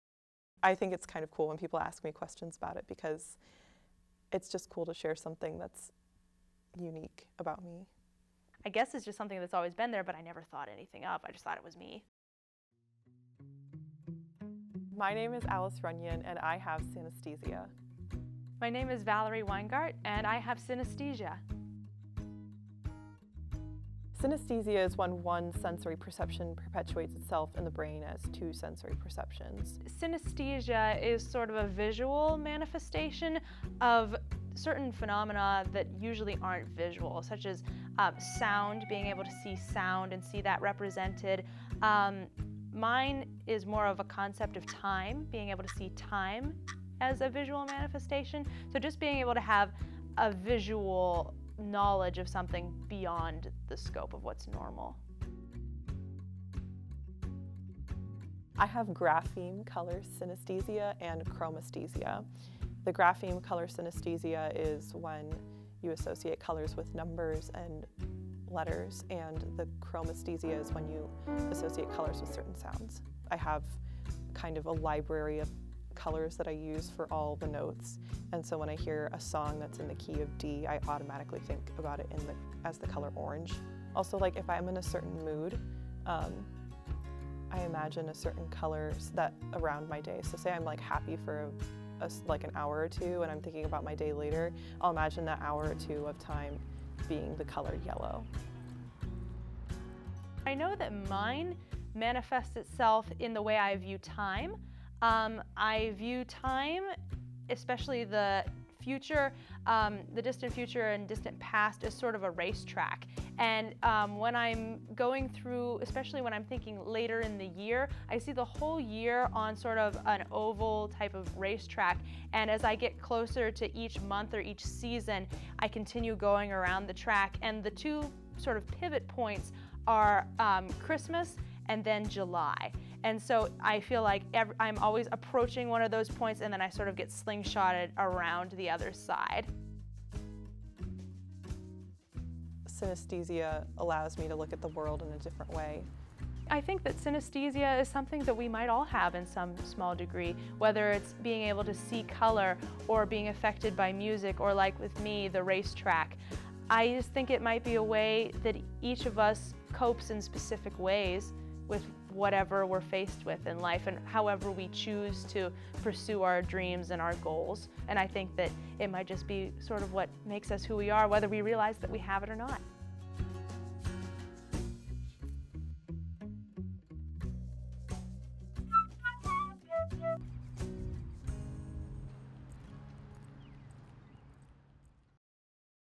Synesthesia: Living Through the Senses
A mini-documentary exploring the neurological condition known as synesthesia.